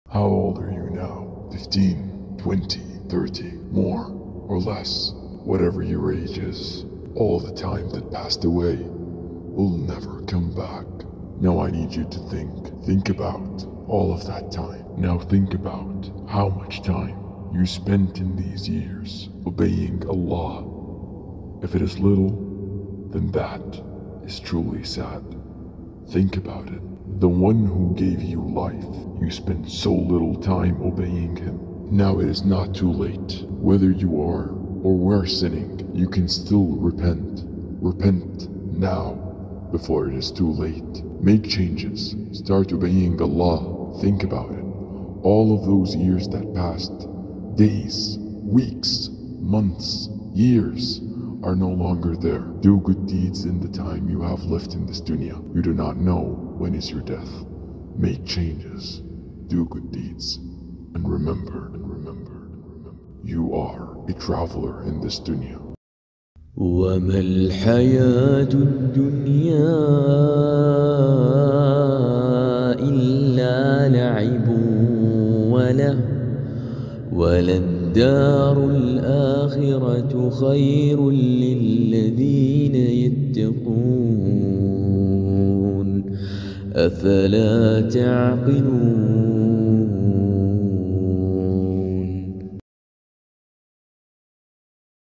How Old Are You? || Islamic Reminder || Percieve Islam أفهم الإسلام
Islamic Reminders:
- Do not take this Dunya as your "HOME" , as we are travellers right here.
- Do not Backbite/Hurt others
- Do not look at the things that Allah took from you , look at the things that Allah gave to you .
- Do not take the muslims as enemies, take the Iblees and his army as your enemies.
- Do not be arrogant
- Do not be a miser, be generous

- Every relationship in this Dunya ends/or is seperated , except your relationship with Allah .

- Every single human being will die so do not take this Dunya as "Home".

- Read the Qur'an

- Do not lose hope in the mercy of Allah .

- Allah created you, fed you , protected you, and did everything for you , so do not disobey Him .